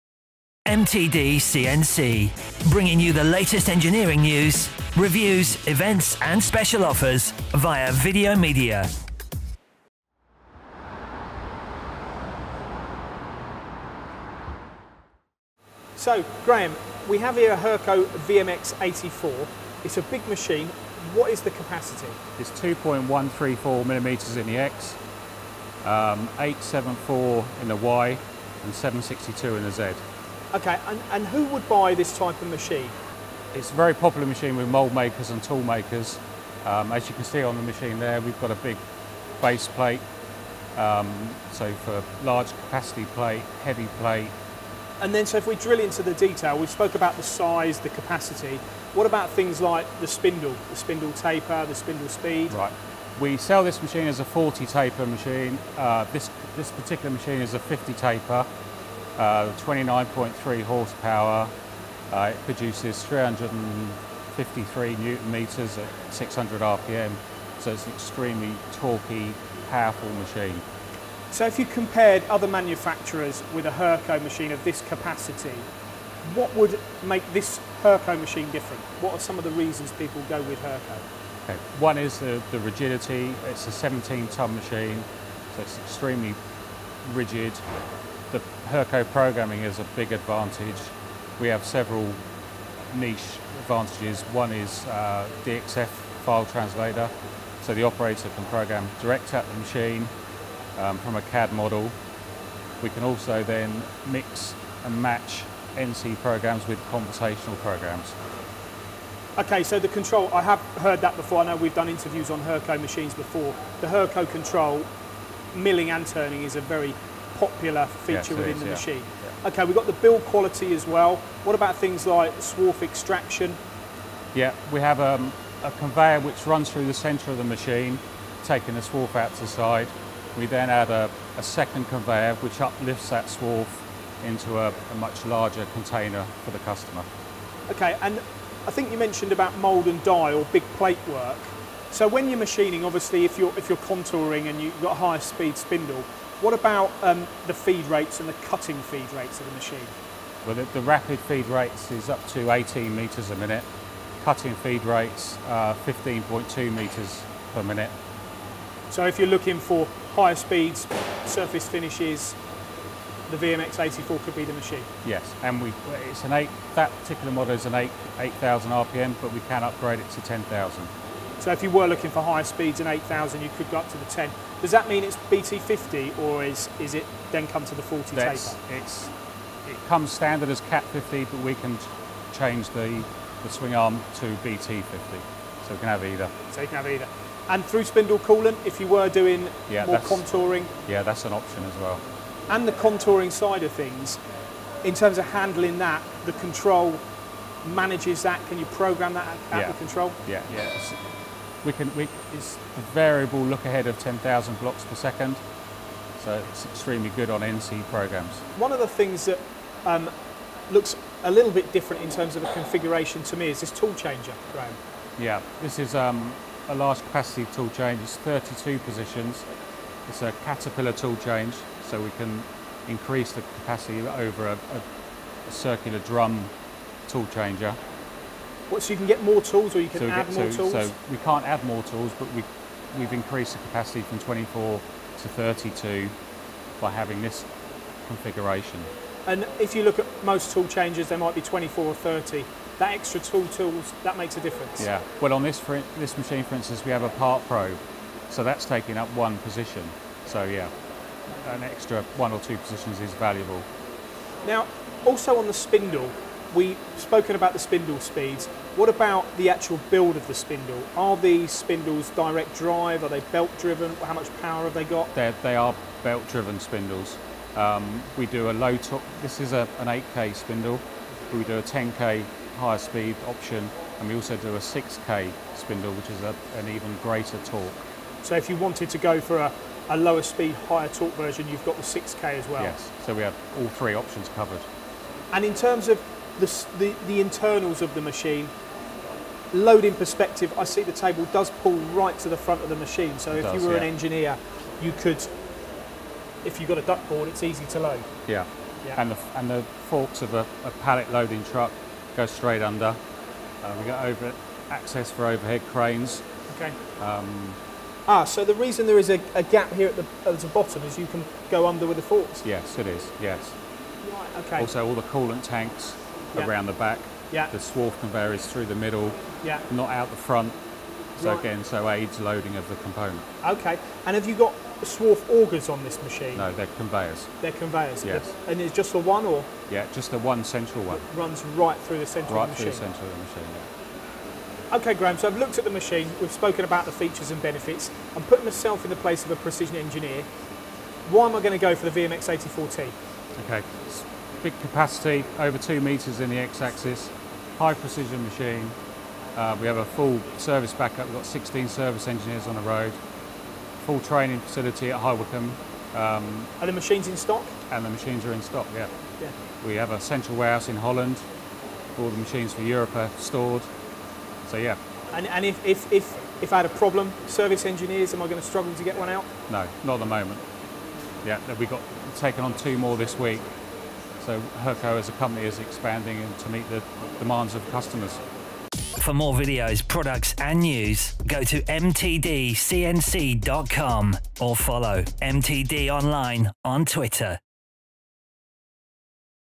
Hurco VMX84 50Ti
The largest and most powerful of our C frame, high performance vertical machining centres. The VMX84 50Ti boasts 2134 x 860 x 760mm travels and a 30HP, 8,000RPM spindle with 353Nm peak torque.